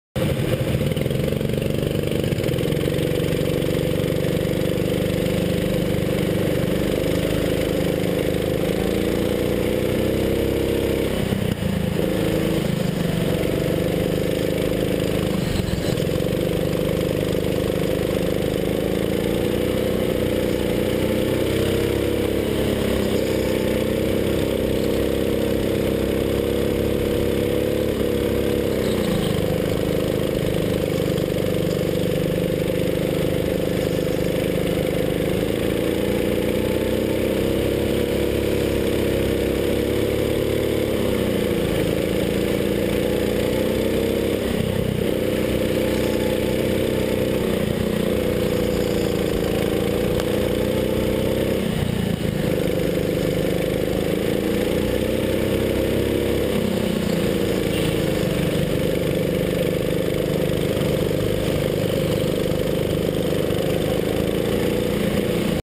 go kart speedstreet mayuyu september 28 2012 Alan's perspective
I'm the one right in front of Alan in the beginning of the video wearing a black shirt